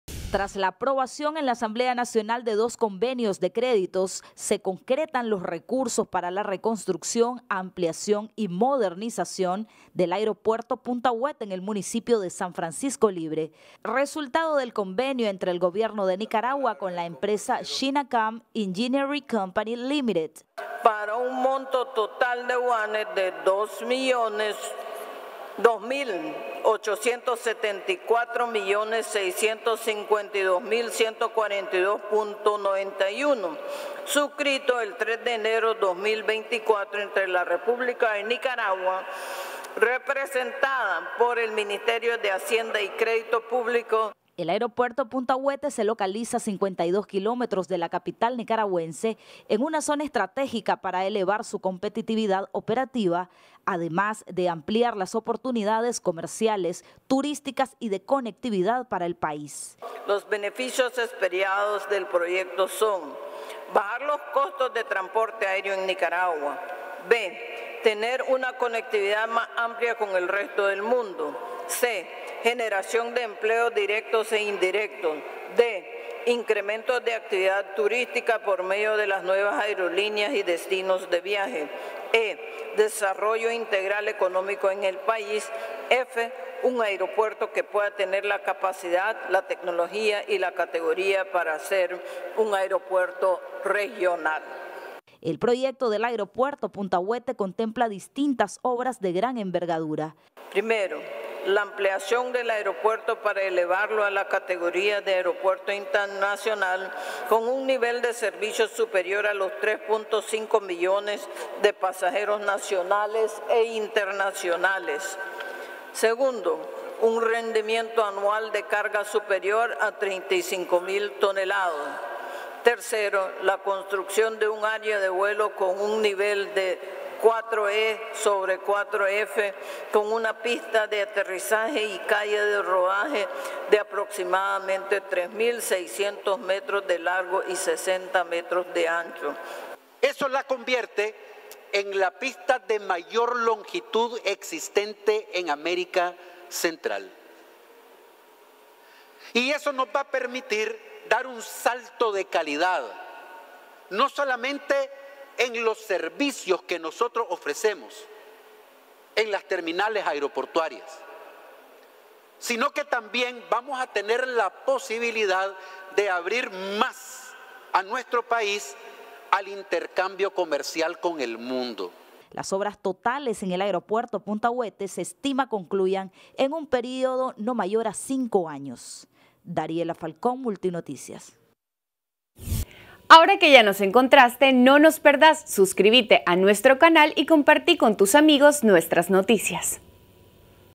Asamblea Nacional aprueba fondos para la modernización del Aeropuerto Internacional Punta Huete
MANAGUA – NICARAGUA / Este viernes 9 de febrero la Asamblea Nacional de Nicaragua ha dado luz verde a la asignación de recursos destinados a la reconstrucción, ampliación y modernización del Aeropuerto Internacional Punta Huete, ubicado en el municipio de San Francisco Libre.

La aprobación se llevó a cabo mediante el respaldo mayoritario de dos convenios de crédito firmados entre el Gobierno de Nicaragua y la destacada empresa china, Camc Engineering Co., Ltd, de la República Popular China.

El proyecto, a cargo del Ministerio de Transporte e Infraestructura (MTI), busca revitalizar y potenciar las capacidades del Aeropuerto Punta Huete, ubicado estratégicamente a 52 kilómetros de Managua